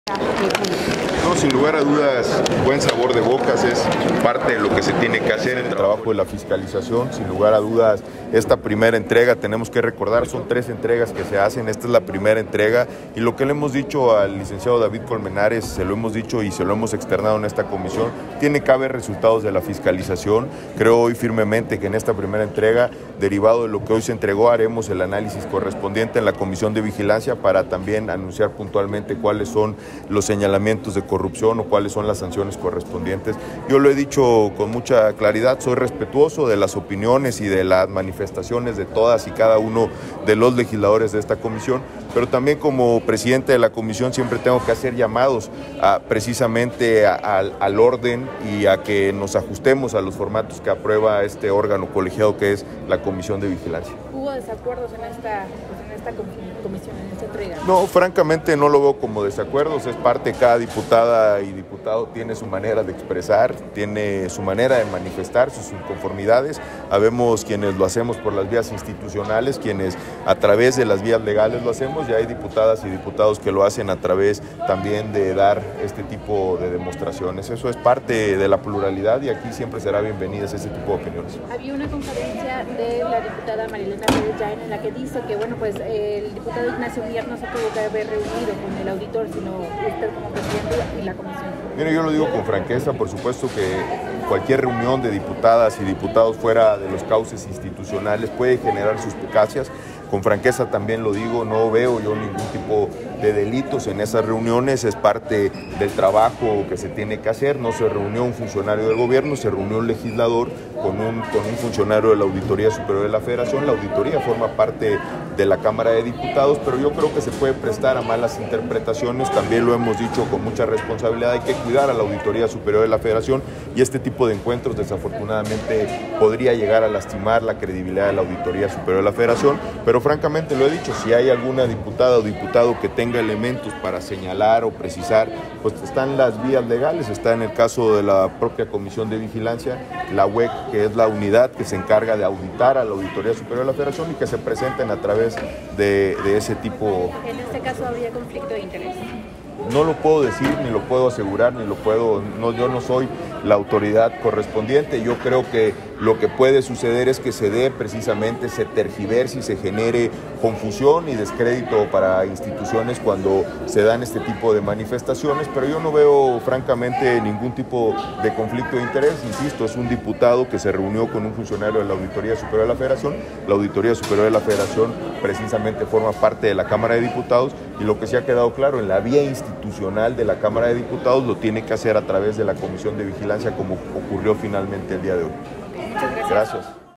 Entrevista | Dip. Pablo Angulo Briceño | Pdte. de la Comisión de Vigilancia de la ASF | 30/06/2023
“Tiene que haber resultados de la fiscalización” de la Cuenta Pública 2022: Pablo Guillermo Briceño

⚫Se hace la Primera Entrega del Informe del Resultado de la Fiscalización Superior de la Cuenta Pública 2022 a la Comisión de Vigilancia de la ASF: Seguramente Resultará en Corrupción y Sanciones

Palacio Legislativo, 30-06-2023 (Notilegis).– El diputado Pablo Guillermo Briceño Angulo (PRI), presidente de la Comisión de Vigilancia de la Auditoría Superior de la Federación, aseguró que la Primera Entrega del Informe del Resultado de la Fiscalización Superior de la Cuenta Pública 2022 informaría sobre corrupción y las sanciones correspondientes.

“Y lo que le hemos dicho al licenciado David Colmenares, auditor superior de la Federación, se lo hemos externado en esta Comisión: tiene que haber resultados de la fiscalización. Creo hoy firmemente que, en esta primera entrega, derivado de lo que hoy se entregó haremos el análisis correspondiente en la Comisión de Vigilancia, para también anunciar puntualmente cuáles son los señalamientos de corrupción o cuáles son las sanciones correspondientes”, aseguró en declaraciones a representantes de medios de comunicación, al término de la reunión de la instancia legislativa.

El diputado aseguró que “esto es parte de lo que se tiene que hacer, es el trabajo de la fiscalización. Ésta es la primera de tres entregas que se hacen.”

Cuestionado sobre los señalamientos de la diputada Inés Parra en el desarrollo de la reunión, dijo: “Soy respetuoso de las opiniones y de las manifestaciones de todos y cada uno de los legisladores de esta comisión, pero también como presidente de esta comisión, siempre tengo que hacer llamados, precisamente, al orden y que nos ajustemos a los formatos que aprueba este órgano colegiado que es la comisión de vigilancia”.

Briceño Angulo afirmó que no hubo desacuerdos en la reunión de entrega: “francamente no lo veo como desacuerdos. Es parte de, cada diputada y diputado tiene su manera de expresar, su manera de manifestar sus inconformidades, habemos quienes lo hacemos a través de las vías institucionales, a través de las vías legales lo hacemos, y hay diputadas y diputados que lo hacen también a través de dar este tipo de demostraciones. Eso es parte de la pluralidad y aquí siempre serán bienvenidas ese tipo de demostraciones y de opiniones”.

Asimismo, interrogado sobre el encuentro del diputado Ignacio Mier Velazco, coordinador del Grupo Parlamentario de Morena, con el auditor en un restaurante de la Ciudad de México recientemente, respondió: “Lo digo con franqueza, por supuesto que cualquier reunión de diputadas y diputados fuera de los cauces institucionales, puede generar suspicacias. Con franqueza también lo digo: no veo ningún tipo de delitos en esas reuniones, es parte del trabajo que se tiene que hacer, puesto que no se reunió un funcionario de gobierno, se reunió un legislador con un funcionario de la Auditoría Superior de la Federación, pero creo que se puede prestar a malas interpretaciones. También lo hemos dicho con mucha responsabilidad: hay que cuidar a la Auditoría Superior de la Federación, y este tipo de encuentros podría llegar a lastimar la autoridad de la ASF.

“Lo que puede suceder es que se tergiverse y se genere confusión y descrédito para instituciones cuando se dan este tipo de manifestaciones, pero no veo, francamente, ningún tipo de conflicto de interés. Repito, es un diputado que se reunió con un funcionario de la Auditoría Superior de la Federación, y ésta precisamente forma parte de la Cámara de Diputados y, lo que sí quedó claro: en la vía institucional lo tiene que hacer a través de la Comisión de Vigilancia, como ocurrió finalmente, el día de hoy”.

Agregó que si hay alguna diputada o diputado que tenga elementos para señalar o precisar algo al respecto, están las vías legales, la UEC, que es la Unidad que se encarga de auditar a la Auditoría Superior de la Federación.

JCHW